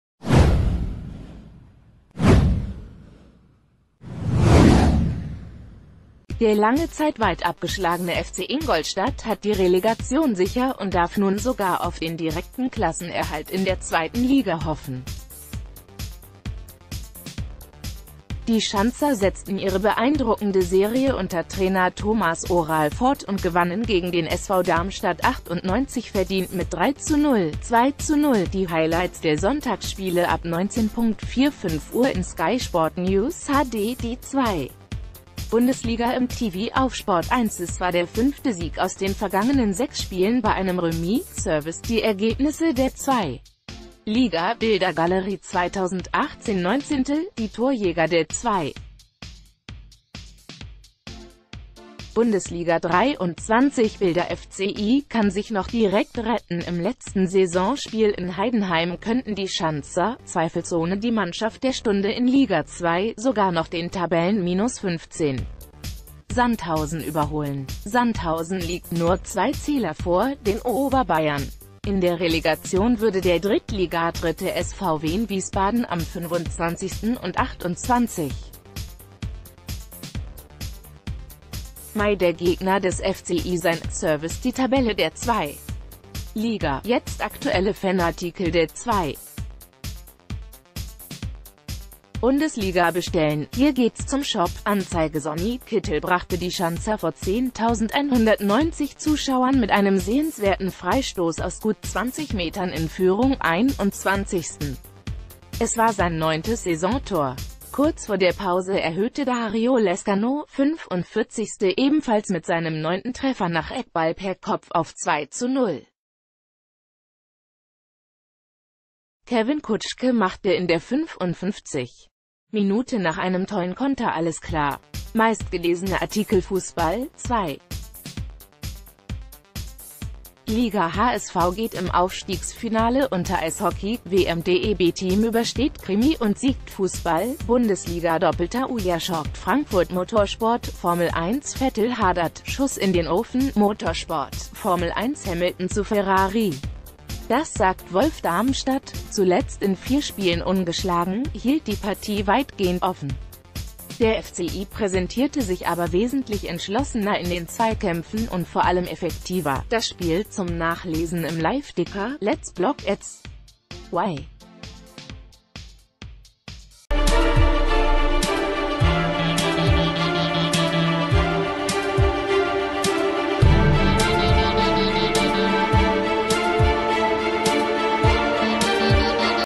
VIDEO: 2. Bundesliga: FC Ingolstadt 04 besiegt SV Darmstadt 98, Relegation sicher
Der lange Zeit weit abgeschlagene FC Ingolstadt hat die Relegation sicher und darf nun sogar auf den direkten Klassenerhalt in der 2. Liga hoffen. Die Schanzer setzten ihre beeindruckende Serie unter Trainer Tomas Oral fort und gewannen gegen den SV Darmstadt 98 verdient mit 3:0 (2:0).Die Highlights der Sonntagsspiele ab 19.45 Uhr in Sky Sport News HD - Die 2. Bundesliga im TV auf SPORT1 Es war der fünfte Sieg aus den vergangenen sechs Spielen bei einem Remis. (Service: Die Ergebnisse der 2. Lig...